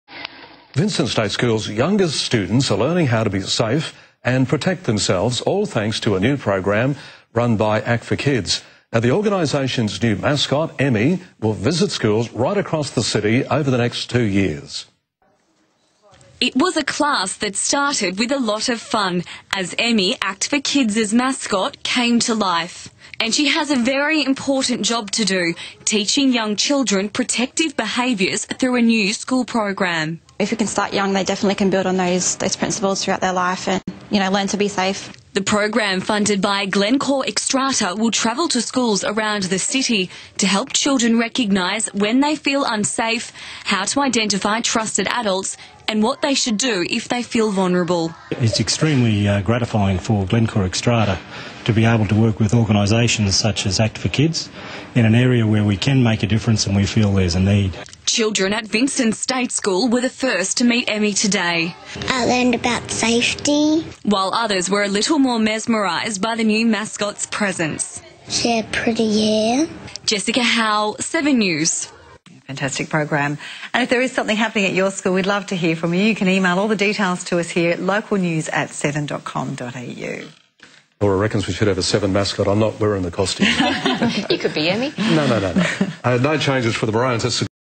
Act for Kids - Learn to BE SAFE with Emmy
The launch of the Learn to BE SAFE with Emmy protective behaviours program for Townsville's grade one students, all thanks to the generous funding of Xstrata Glencore.